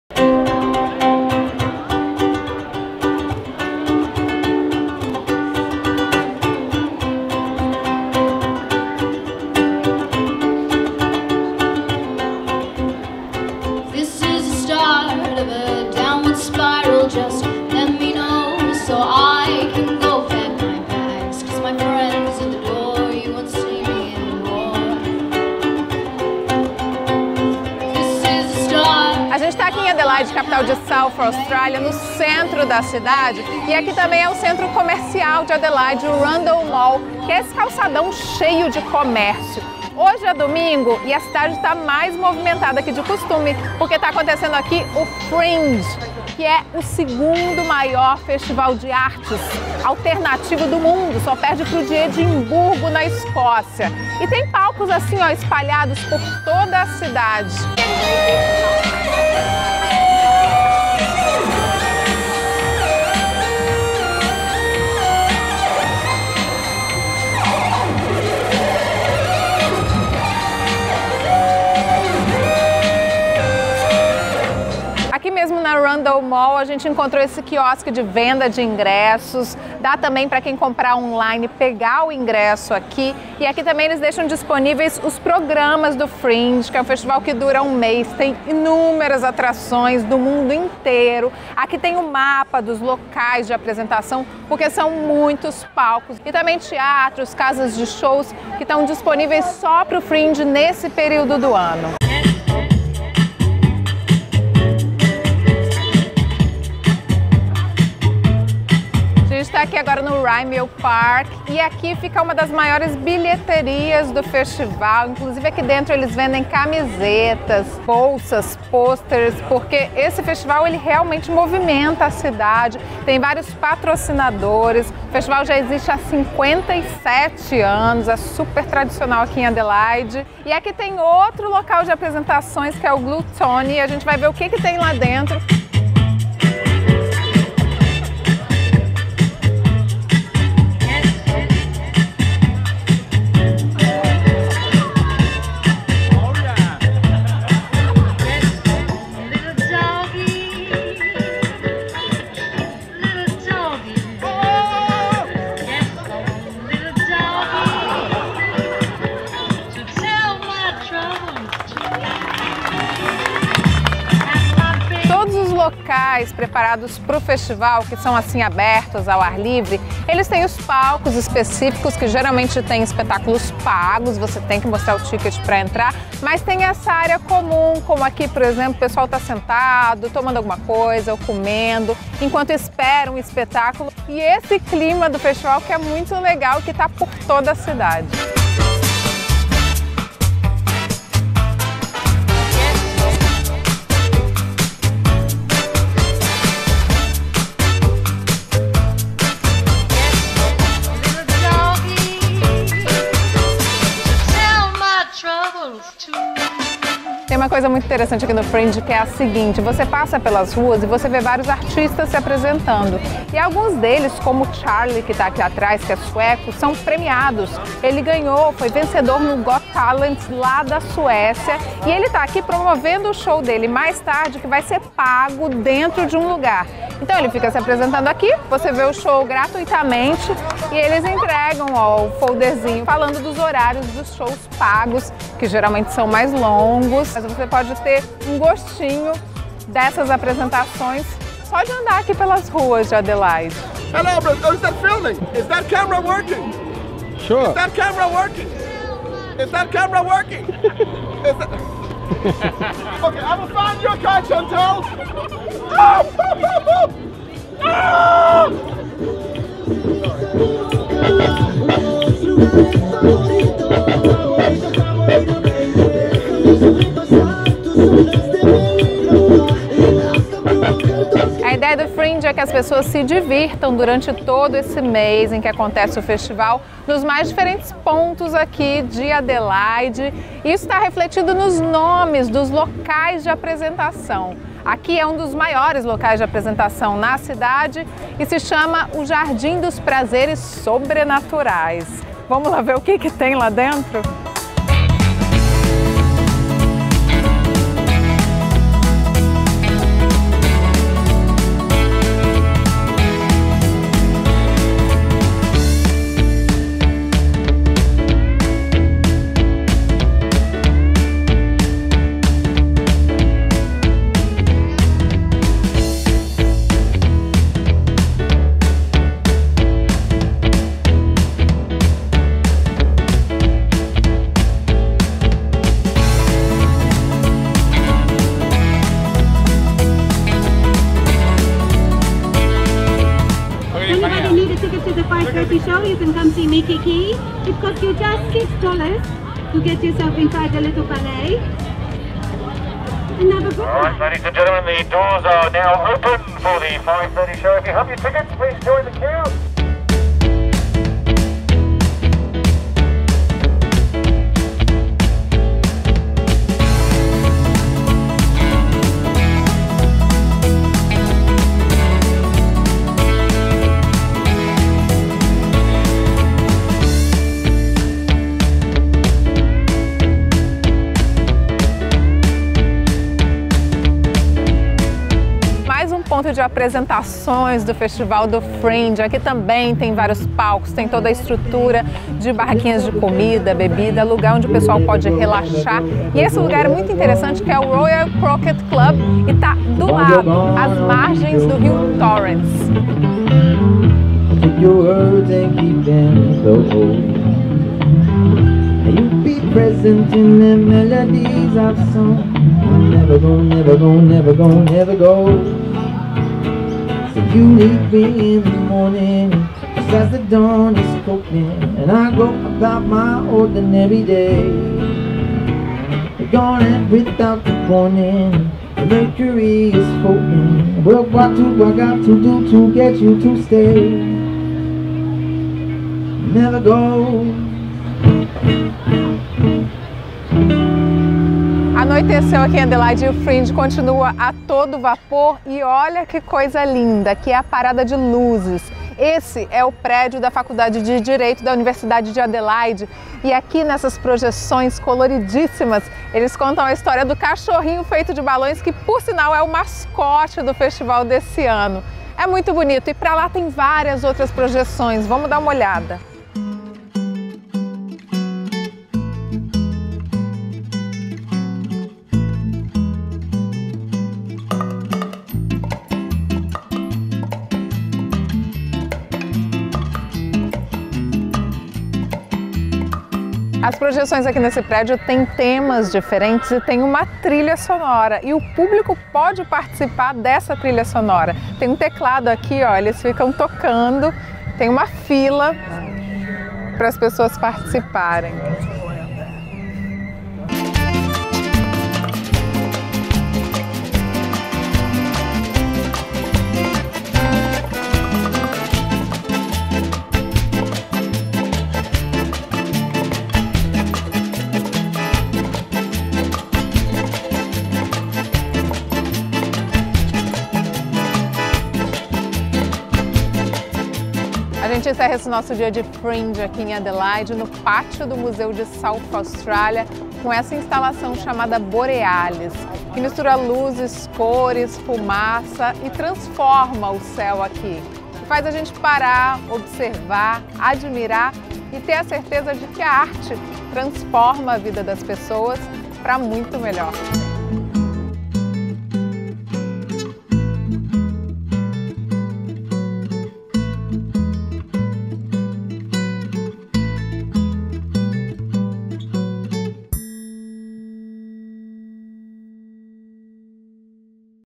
Adelaide Fringe | Intercâmbio na Austrália
Adelaide, capital de South Australia, sedia o segundo maior Fringe do mundo. Um festival cultural que recebe artistas de vários países e durante um mês, sempre no começo do ano, transforma a cidade num grande palco com as mais diversas formas de arte. É o que a equipe da WEST 1 TV mostra pra gente nesse vídeo. :)

Para mais informações sobre intercâmbio na Austrália, Irlanda e Nova Zelândia, entre em contato com a gente.
Será um prazer embarcar nessa jornada com você.
                    WEST 1 Irlanda
                    WEST 1 Nova Zelandia